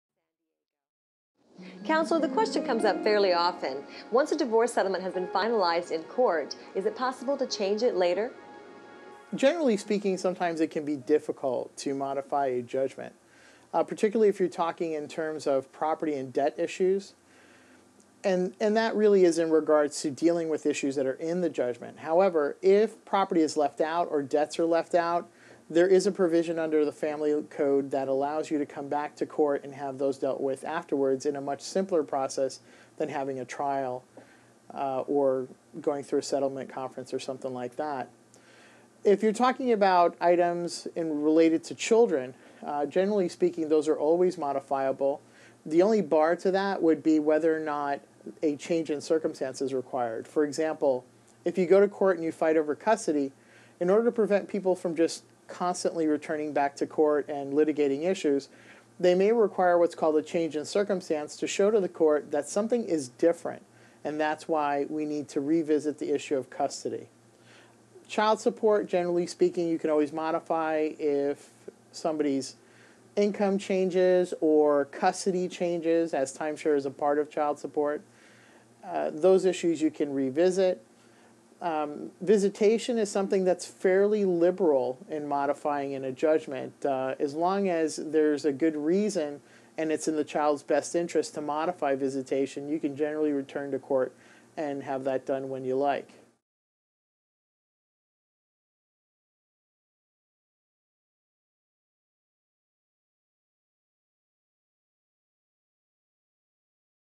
Legal Advice, Court Order modification, Stephen Rocha, Custody, San Diego
Legal advice is given by A. Stephen Rocha in the episode of the Trust Factor legal series.
Stephen Rocha answers the question: Can a judgment or court order be modified in a divorce case?
Stephen Rocha discusses change in circumstances, family code, visitation and divorce settlements in this segment.